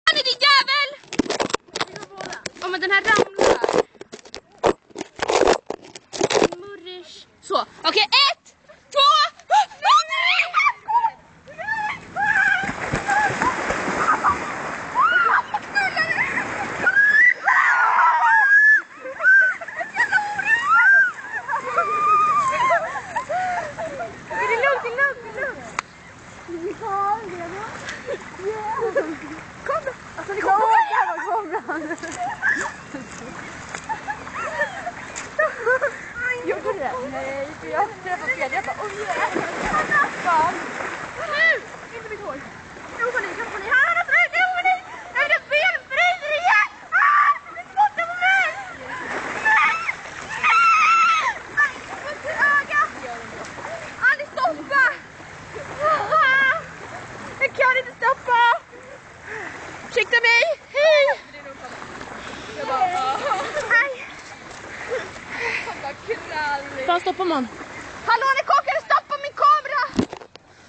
imsobloginthis
annai och jag badar i sätra badet (: